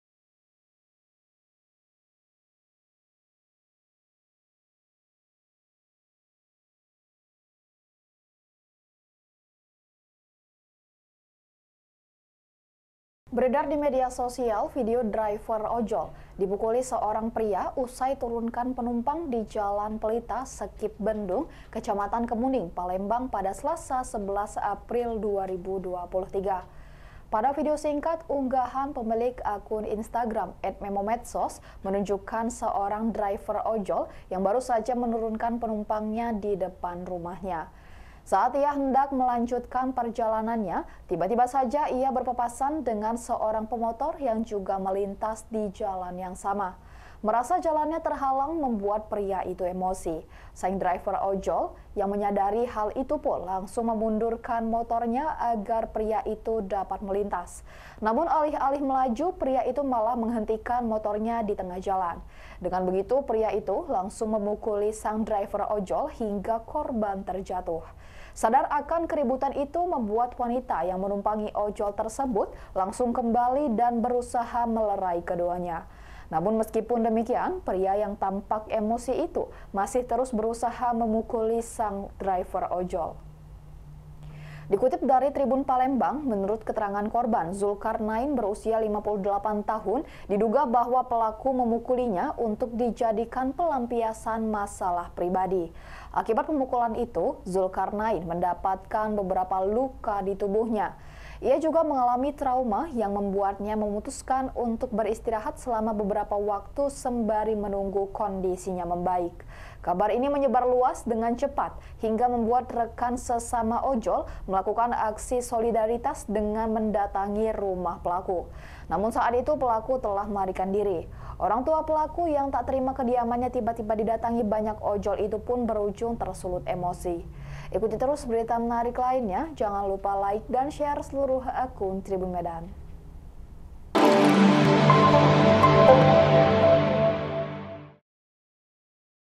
Driver Ojol Dipukuli Seorang Pria Usai Turunkan Penumpang
Pada video singkat unggahan pemilik akun Instagram @memomedsos menunjukkan seorang driver ojol yang baru saja menurunkan penumpangnya di depan rumahnya.

Saat ia hendak melanjutkan perjalanannya, tiba-tiba saja ia berpapasan dengan seorang pemotor yang juga melintas di jalan yang sama.

Merasa jalannya terhalang membuat pria itu emosi.

Sang driver ojol yang menyadari hal itu pun langsung memundurkan motornya agar pria itu dapat melintas.

Namun alih-alih melaju, pria itu malah menghentikan motornya di tengah jalan.

Dengan begitu pria itu langsung memukuli sang driver ojol hingga korban terjatuh.

Sadar akan keributan itu membuat wanita yang menumpangi ojol tersebut langsung kembali dan berusaha melerai keduanya.

Namun meskipun demikian pria yang tampak emosi itu masih terus berusaha memukuli sang driver ojol.

Dikutip dari Tribun Palembang, menurut keterangan korban, Zulkarnain (58) diduga bahwa pelaku memukulinya untuk dijadikan pelampiasan masalah pribadi.

Akibat pemukulan itu, Zulkarnain mendapatkan beberapa luka di tubuhnya.

Ia juga mengalami trauma yang membuatnya memutuskan untuk beristirahat selama beberapa waktu sembari menunggu kondisinya membaik.

Kabar ini menyebar luas dengan cepat hingga membuat rekan sesama ojol melakukan aksi solidaritas dengan mendatangi rumah pelaku .

Namun saat itu pelaku telah melarikan diri.

Orang tua pelaku yang tak terima kediamannya tiba-tiba didatangi banyak ojol itu pun berujung tersulut emosi.